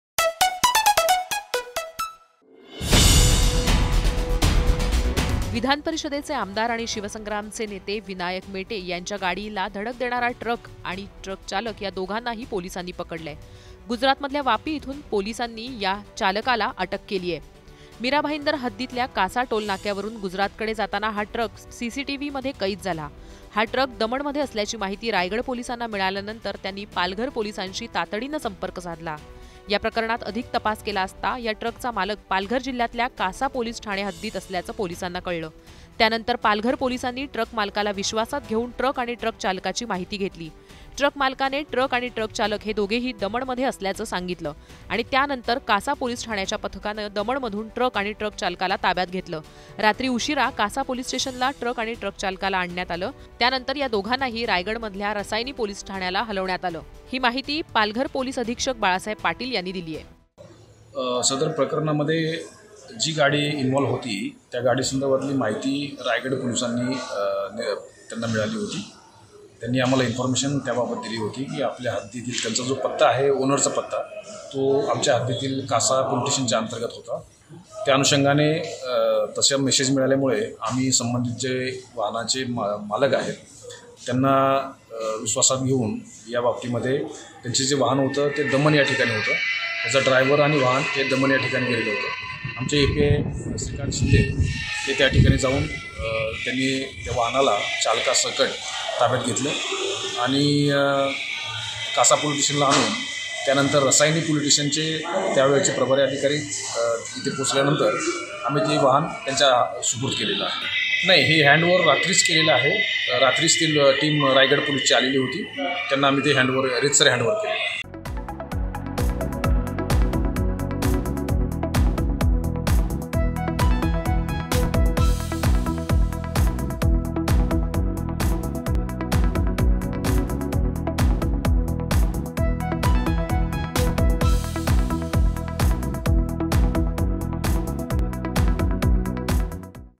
Vinayak Mete death : मेटेंच्या कारच्या अपघाताला कारण ठरलेल्या फरार ट्रक ड्रायव्हरला अटक
शिवसंग्रमाचे नेते आणि मराठा  समाजासाठी संघर्ष करणारे विनायक मेटे यांच्या कारला झालेल्या अपघातात त्याचा मृत्यू झाला. मेटेंच्या कारला धडक बसलेल्या ट्रकचा पत्ता पोलिसांना सापडला. 14 ऑगस्टला रात्री गुजरातमधल्या वापी येथून हा ट्रक आणि ट्रक चालकाला अटक करण्यात आलं. पालघर पोलिसांनी कारवाई करुन ट्रक चालकाला आणि ट्रकला रायगड पोलिसांच्या ताब्यात दिलं.